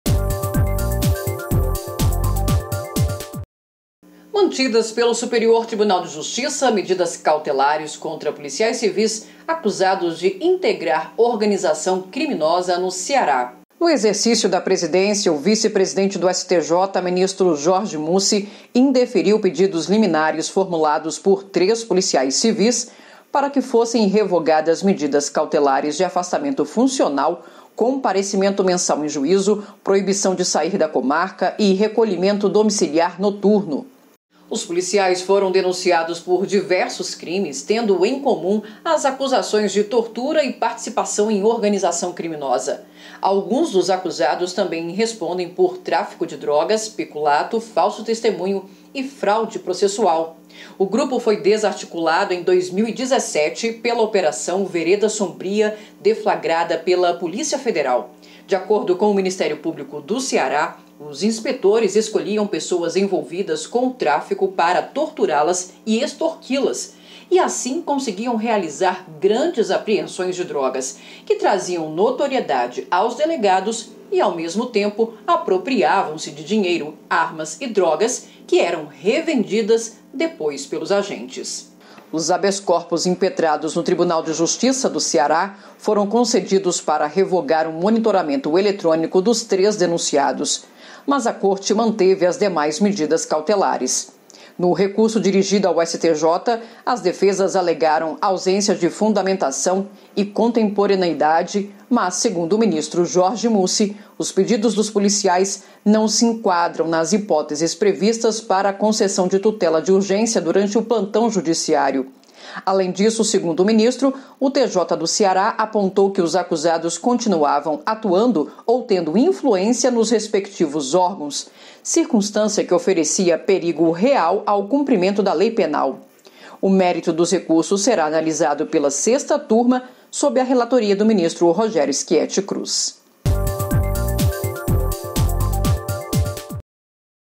Mantidas cautelares para policiais civis acusados de integrar organização criminosa no Ceará
​O ministro Jorge Mussi, vice-presidente do Superior Tribunal de Justiça (STJ), no exercício da presidência, indeferiu pedidos liminares formulados por três policiais civis para que fossem revogadas medidas cautelares de afastamento funcional, comparecimento mensal em juízo, proibição de sair da comarca e recolhimento domiciliar noturno.
RHC 159455
RHC 159461
RHC 159503